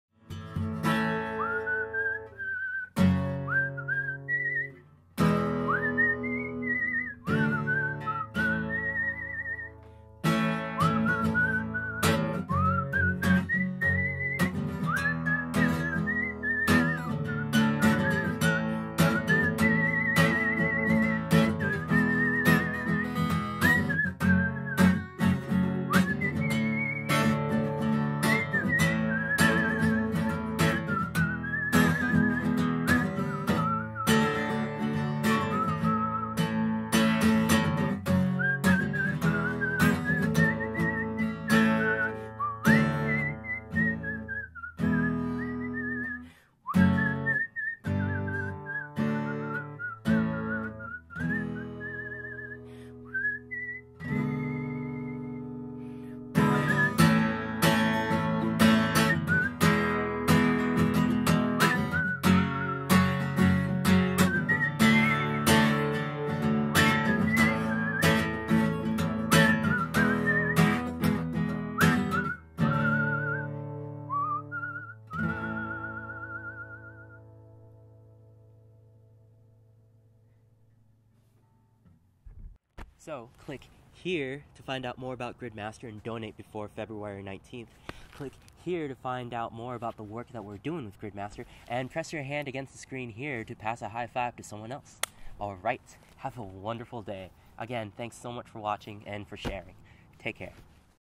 How to use a Grid Master to Draw a Landscape #1
Donate to our Indiegogo campaign here so we can have enough money to manufacture Grid Master so that people around the world can use it!
February 19th 2015 is our deadline!

Usually I'd spend most of the time having to calculate the ratio for the right number of squares that I'd have to use, then I'd have to draw all the squares, and by the end of the painting I'd have to erase any extra squares. Instead with Grid Master I can get right to drawing this landscape of a wonderful summercamp in Buena Vista, Colorado.